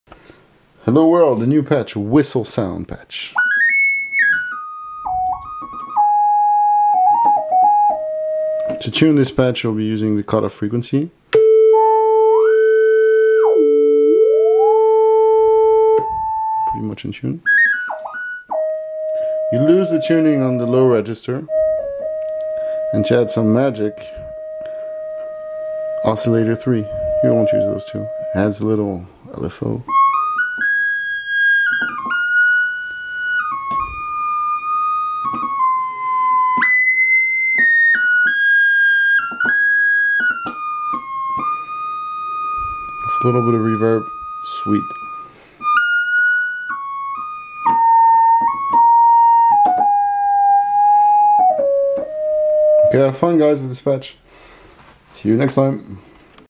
Model D Patch French haunting whistle flute
Mod depth closer to 0 then 1 just a little should do ! Have Fun !